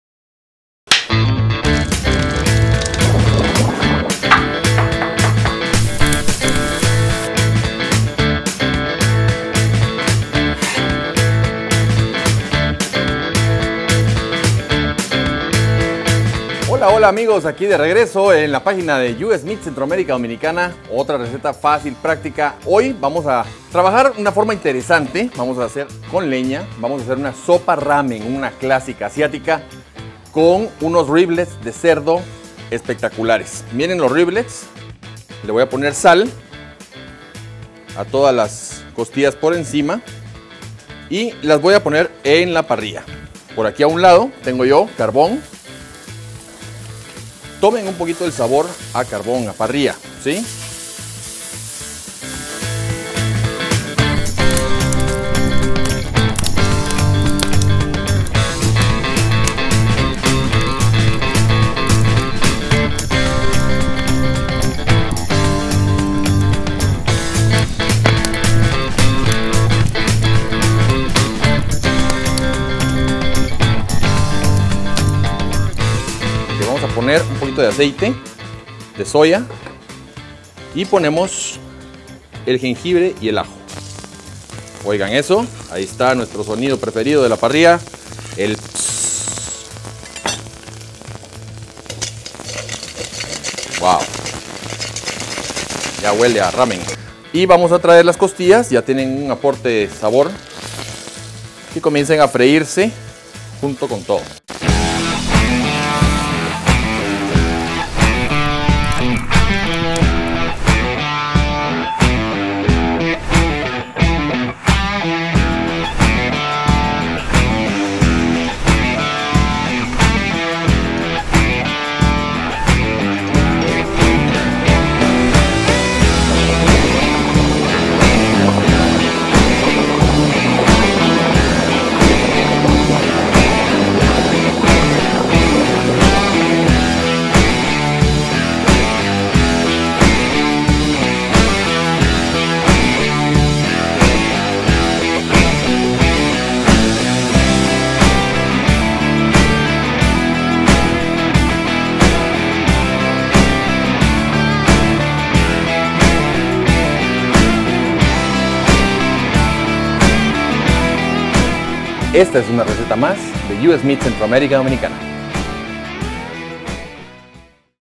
Sopa Ramen con Riblets a la Parrilla
Chef Pablo nos prepara esta deliciosa Sopa Ramen. ¡Te encantará!